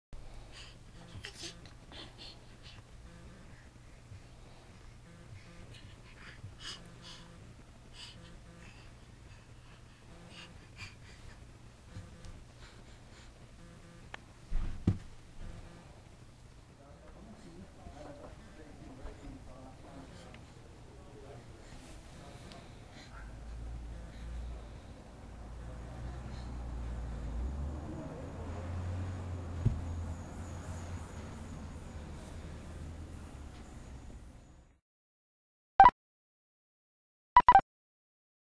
Annika Crawl
Annika crawling to get my slipper.